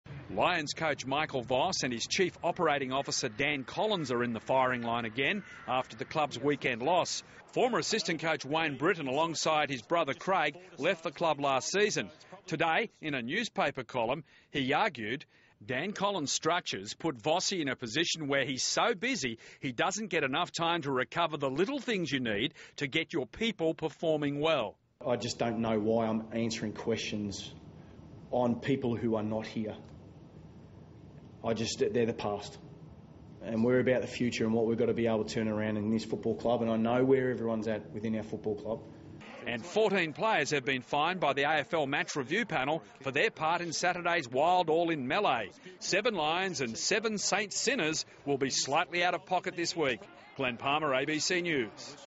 Voss in the firing line after Lions' loss
Brisbane coach Michael Voss and Chief Operating Officer Dan Collins are under more pressure after the club's loss to the Saints.